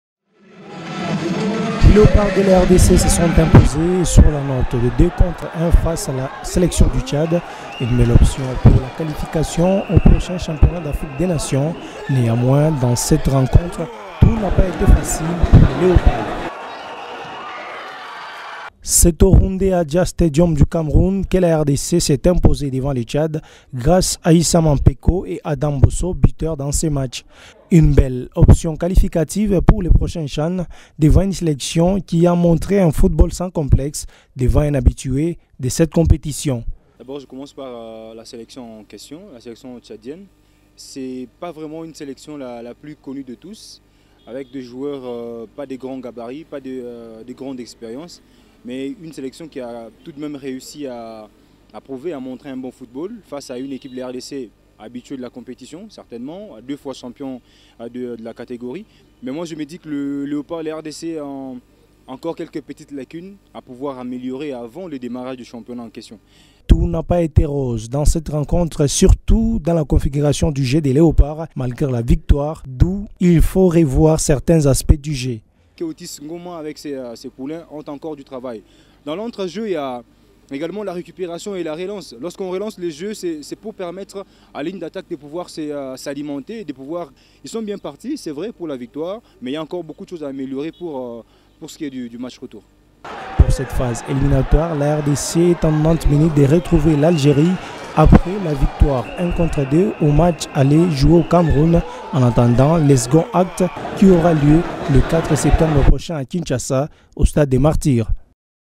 Le retour? La prestation des Léopards au Tchad passée au peigne fin
Belle performance de l'équipe nationale de football (hommes) de la RDC. La prestation des Léopards au Tchad passée au peigne fin.